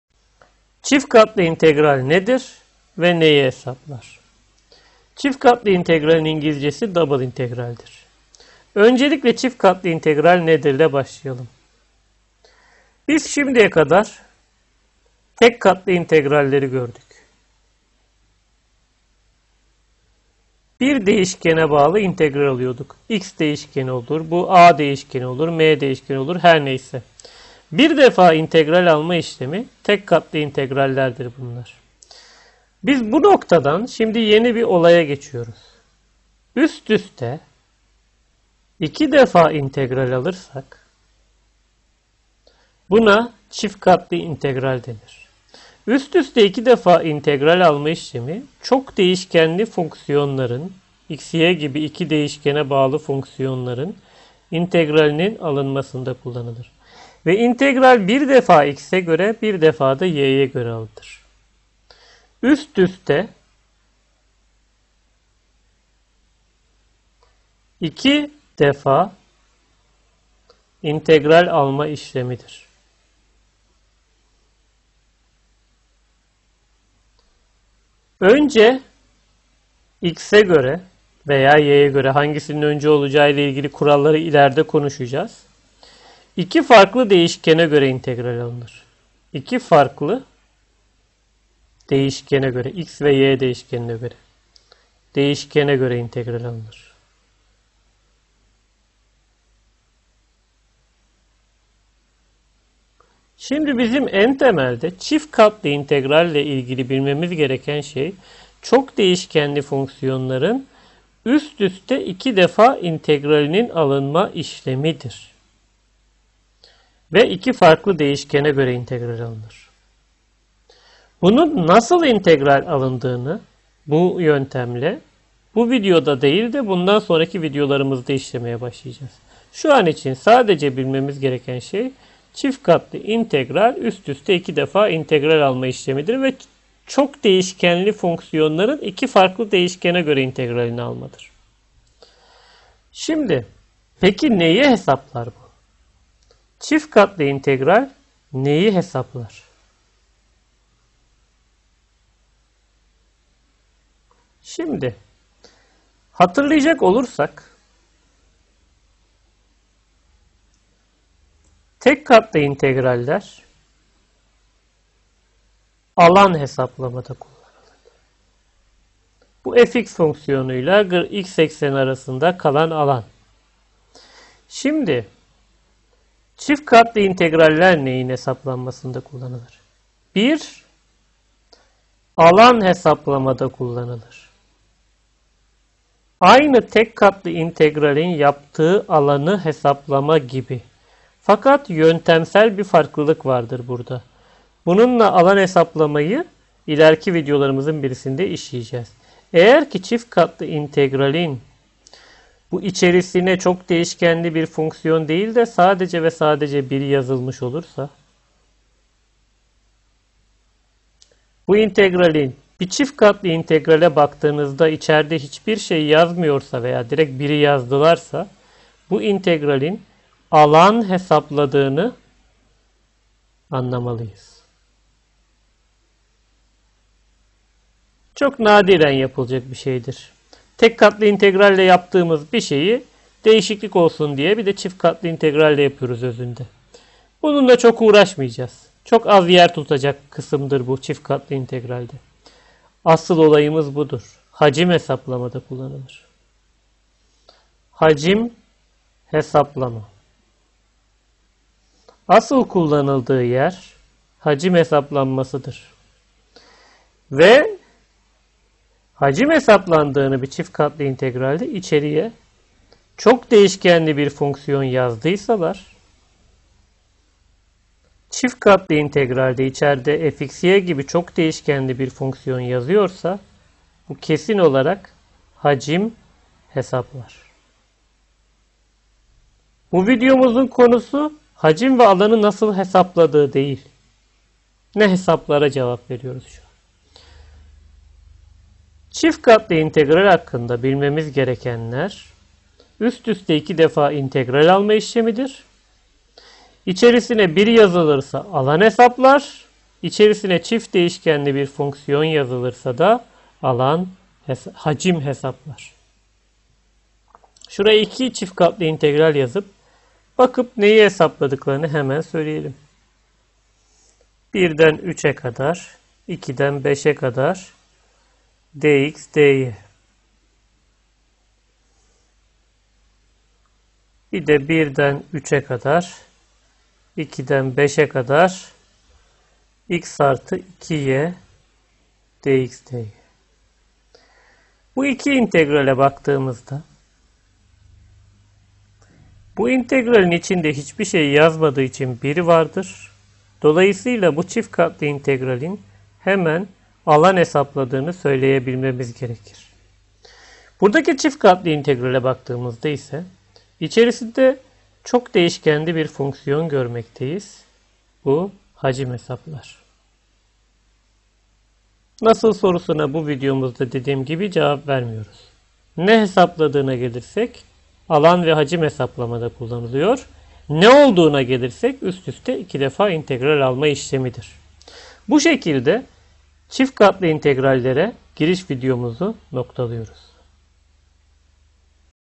Calculus-II : Çift Katlı İntegral Nedir ve Neyi Hesaplar? (Double Integral)
BUders üniversite matematiği derslerinden calculus-I dersine ait " Çift Katlı İntegral Nedir ve Neyi Hesaplar? (Double Integral) " videosudur. Hazırlayan: Kemal Duran (Matematik Öğretmeni) adresinden özgeçmişe ulaşabilirsiniz.